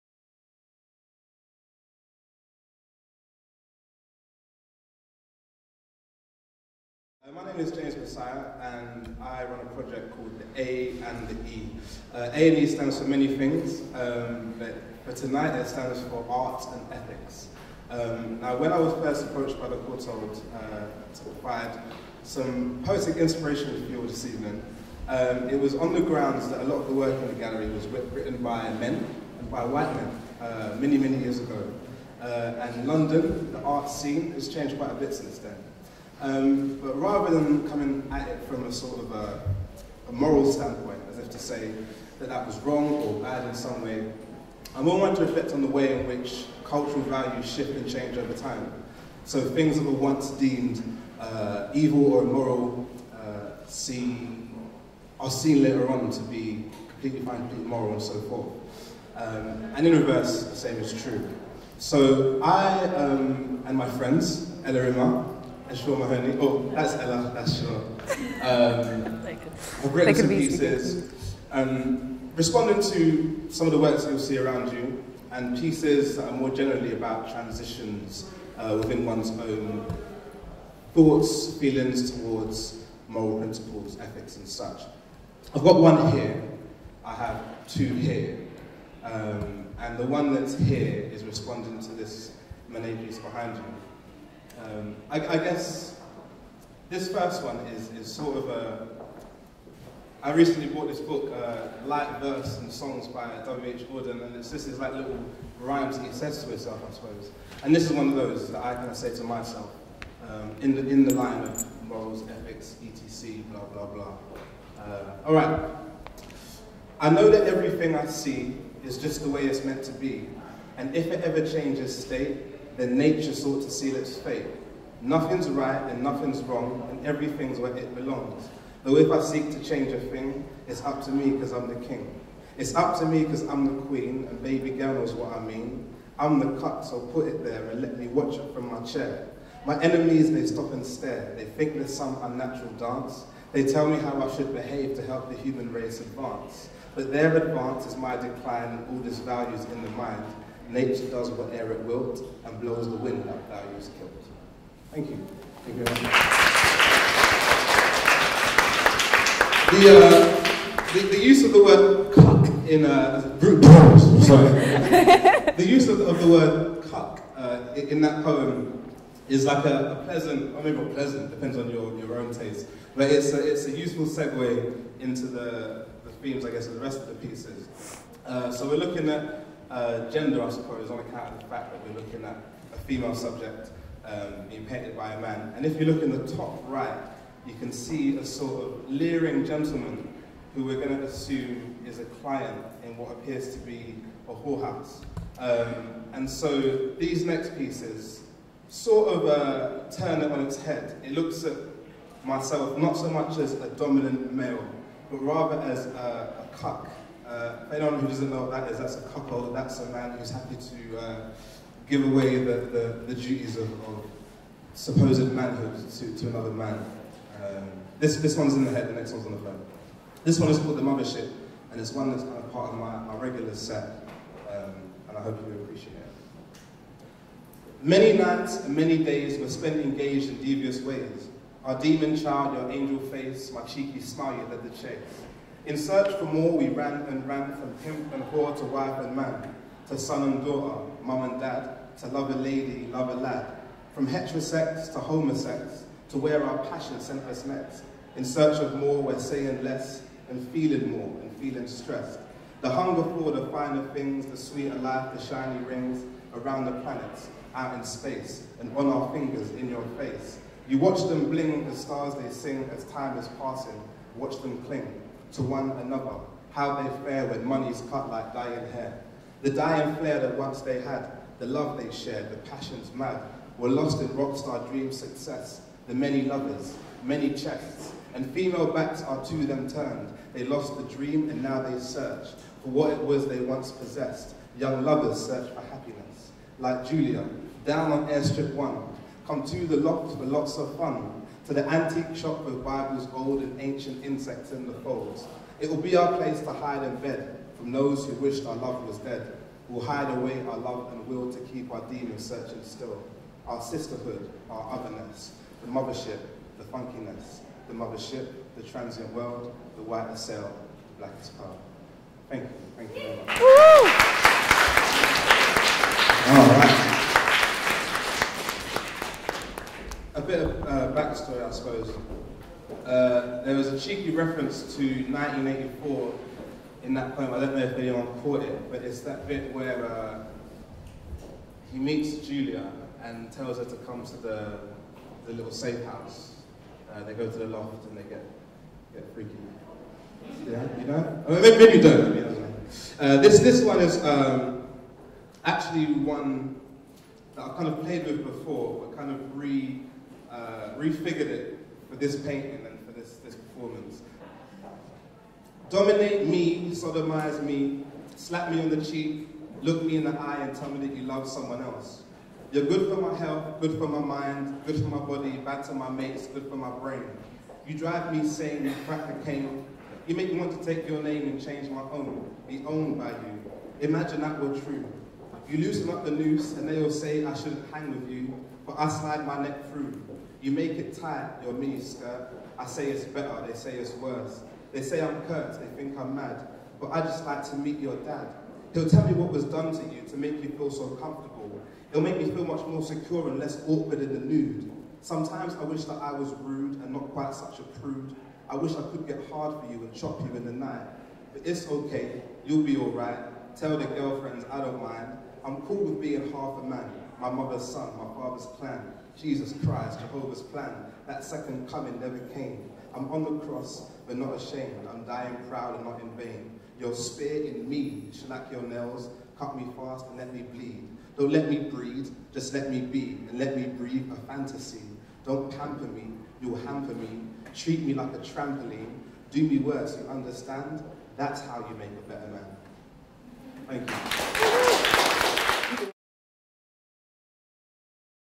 James Massiah @ Res|fest '17
At The Courtauld Institute of Art's Res|fest '17, James Massiah, Poet, responds to highlights from The Courtauld Gallery Collection.